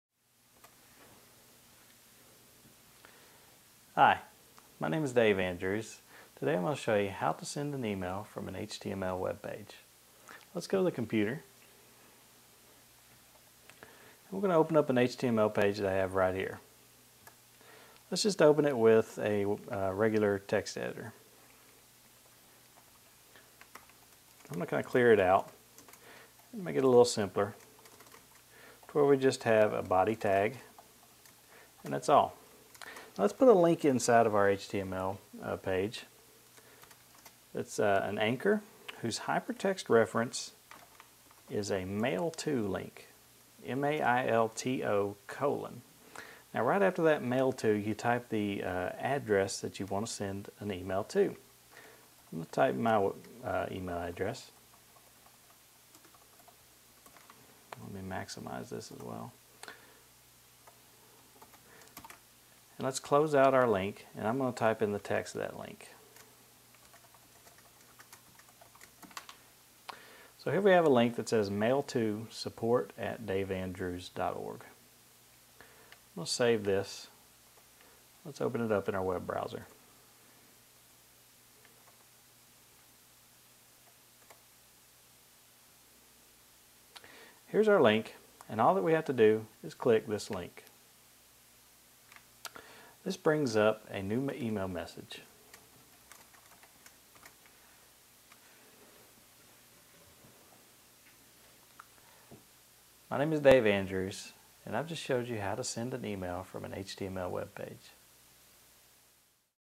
Microsoft Excel, SQL & Internet Tips : How to Send E-mail From an HTML Page
Use a regular text editor to send e-mail from an HTML page with tips from a software developer in this free video on using the Internet.